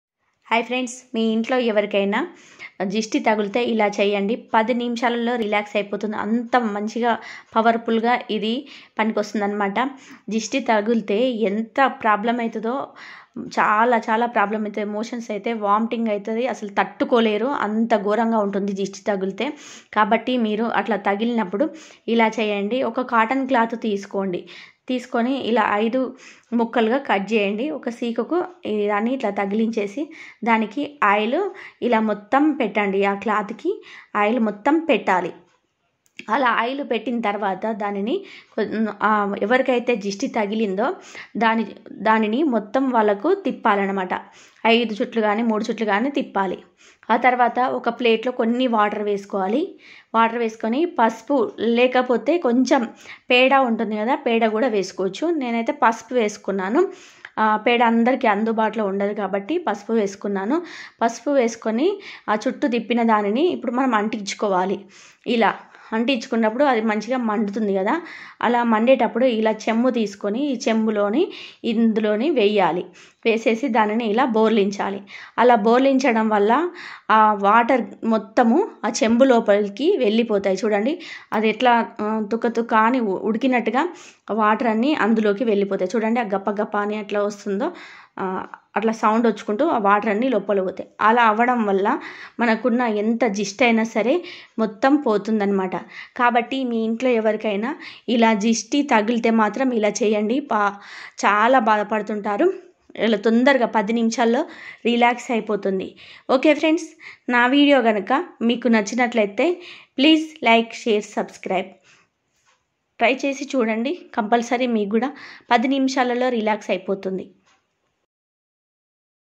మీఇంట్లో ఎవరికి అయిన దిష్టి తగిలితే ఇలా చేయండి 10 నిమిషాల్లో దిష్టి తగ్గుతుంది
దిష్టి ఇలా చేయడం వల్ల 10 నిమిషాల్లో తగ్గుతుందిuseful